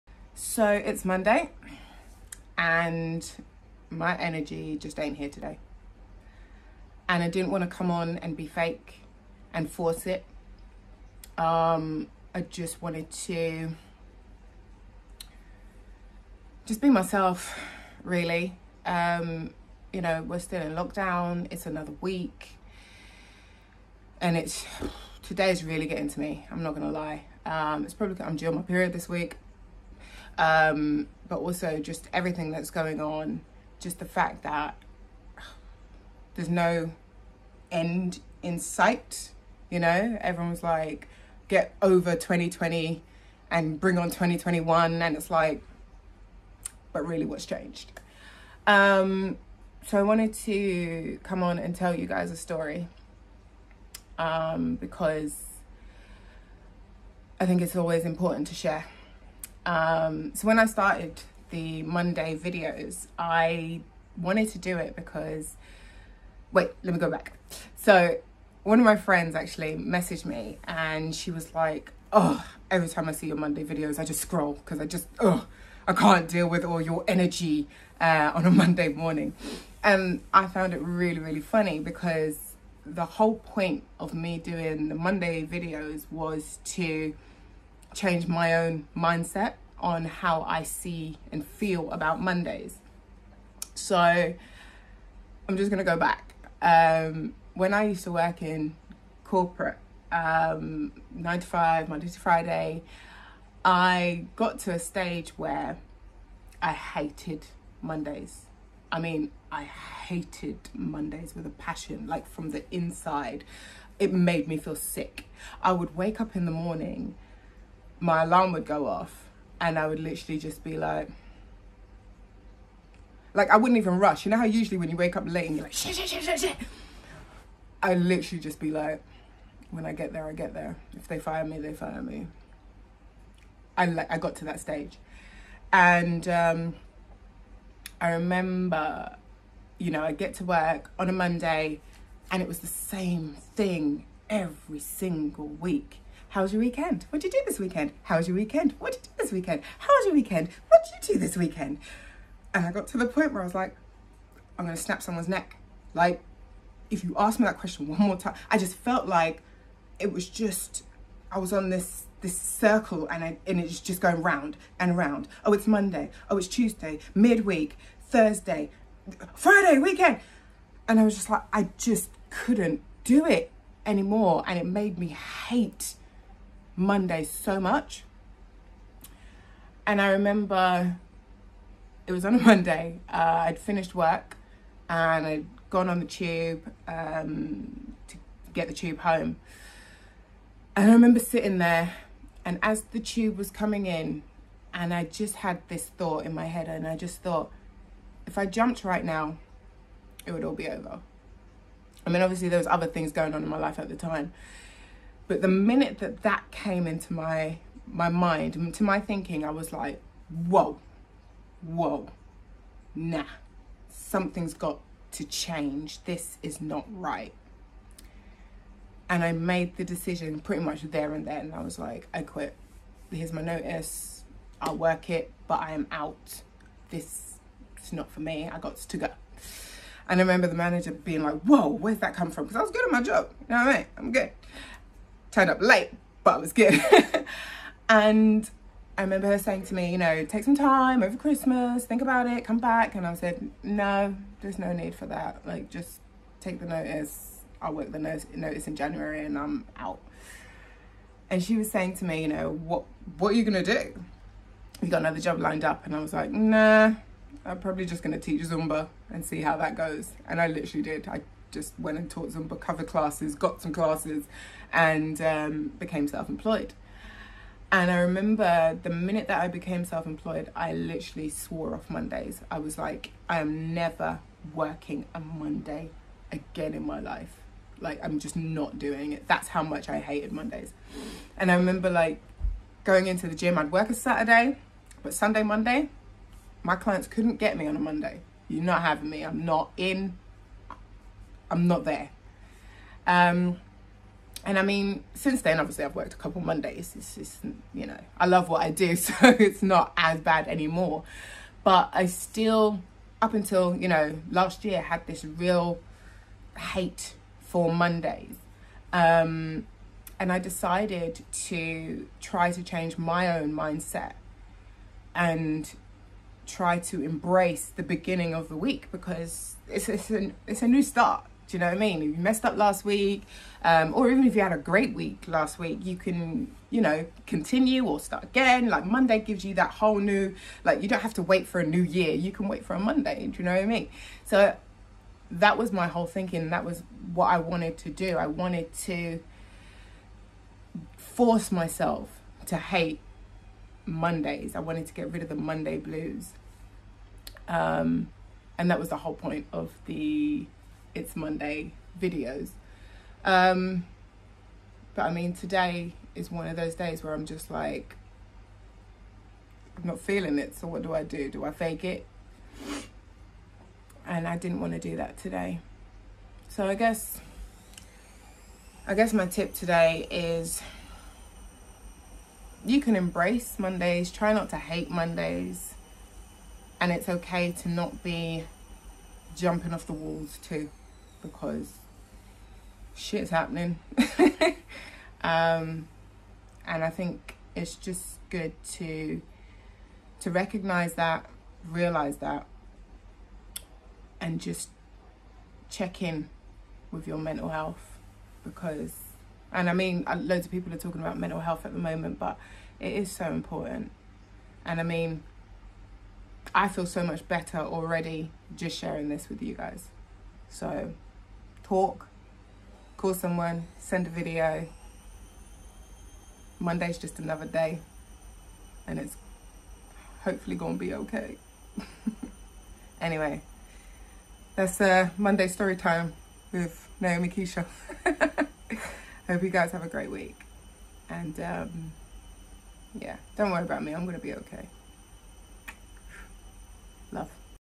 I actually hate Mondays... here’s why.
My Monday videos are actually something I wanted to do to change my own mindset to how I feel about Monday’s... here is why